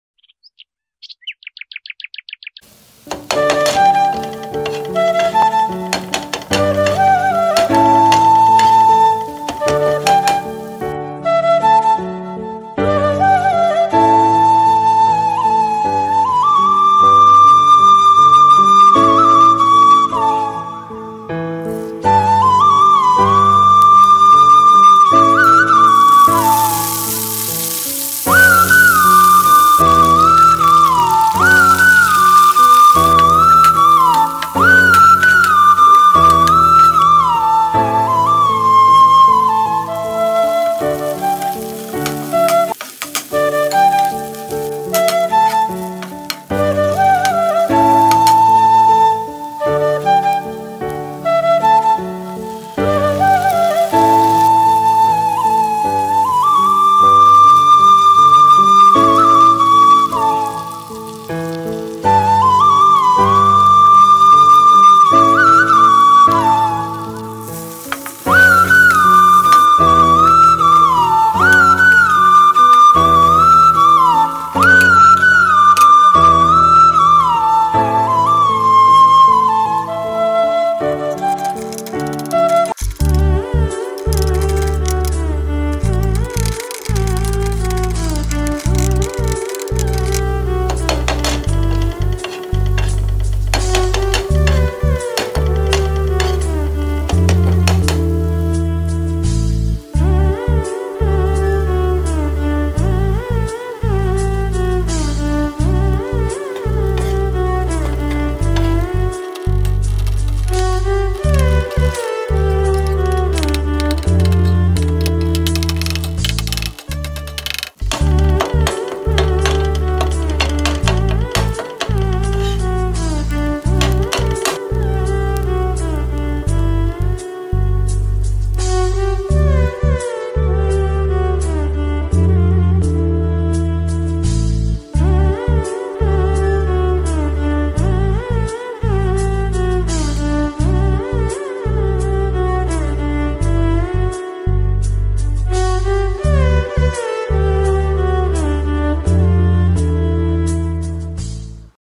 Tasty Madurai Muttai Kothu Idli | Egg Idli Fry | Easy Leftover Idli recipe | Roadside Kothu Idli
This is a tasty and easy recipe which can even be made with leftover Idlis! This dish is famous in Madurai and can be found easily in roadside shops